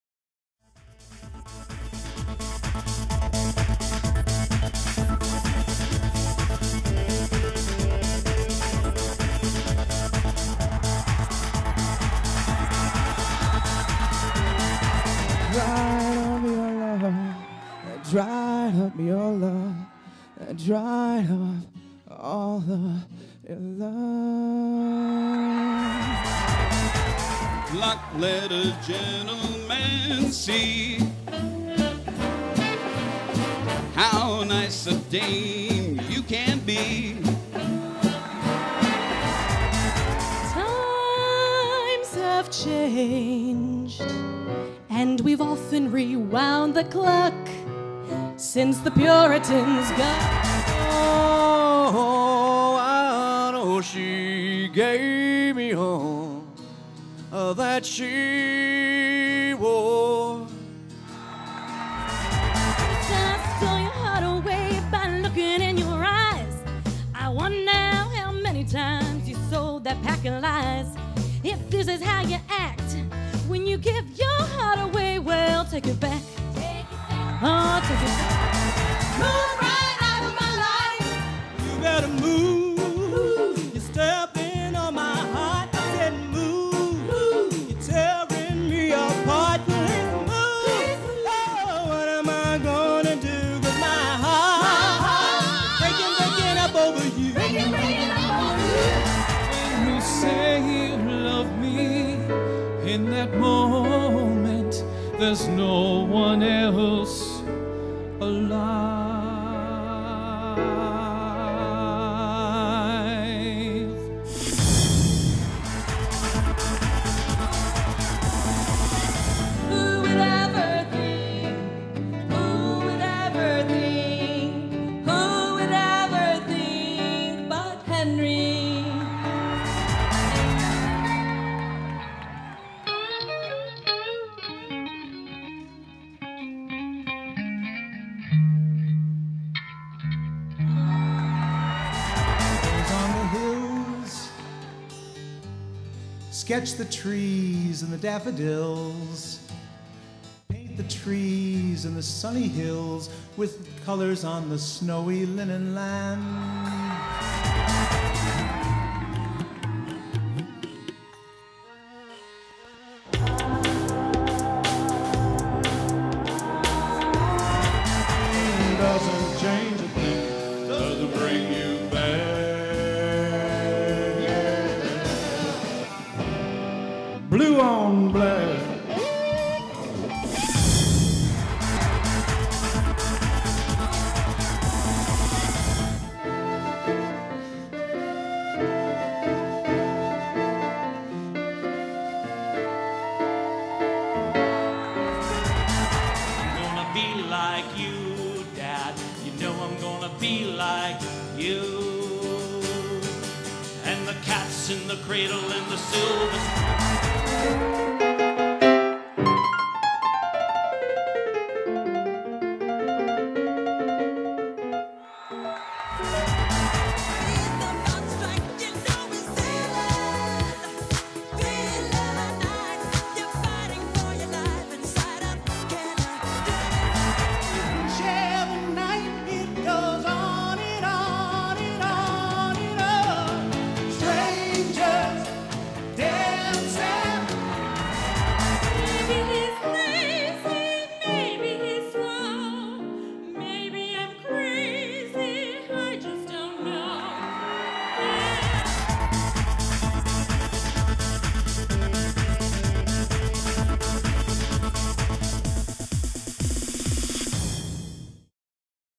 Timken's Got Talent
Recap of round 1 performances in Canton,OH for local United Way event.